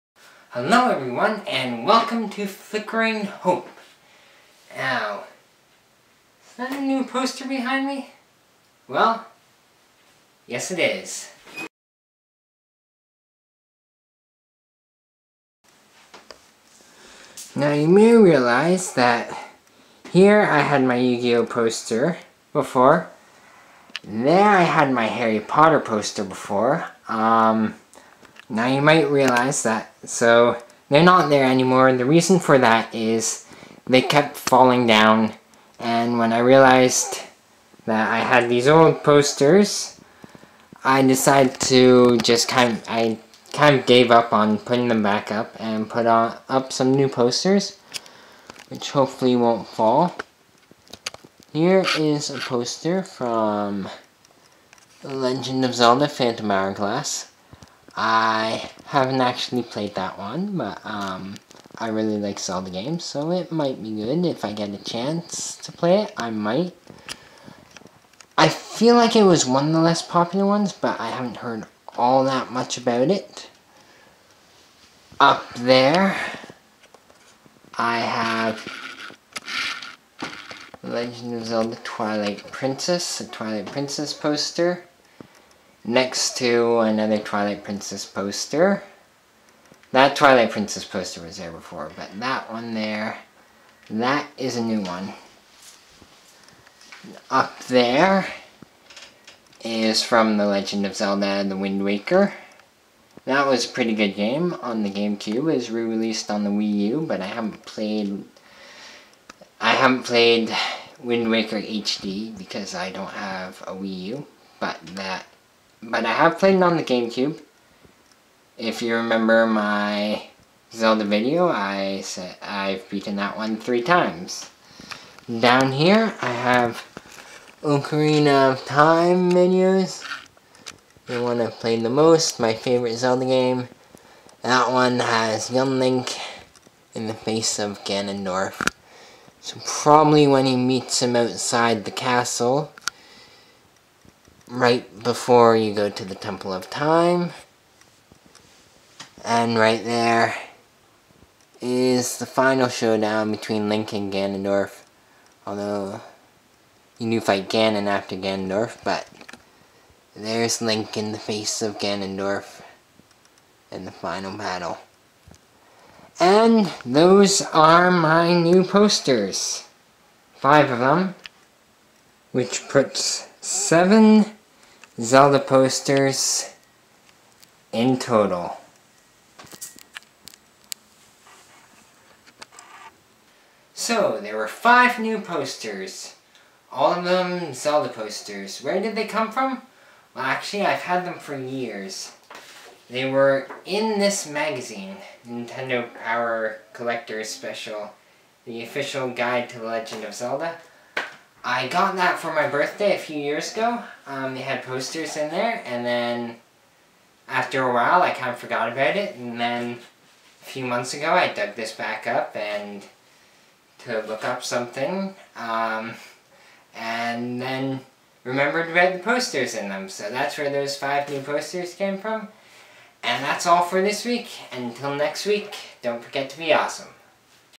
Flickering Hope Episode 23 (New Posters)
My Yugioh and Harry Potter posters kept falling down and I finally decided to give up on putting them back up. What I did instead was put up some new posters in their place as well as fill some empty space on the wall. I may say new posters but technically they're not new. I have had them for a few years but this is the first time they've been put up.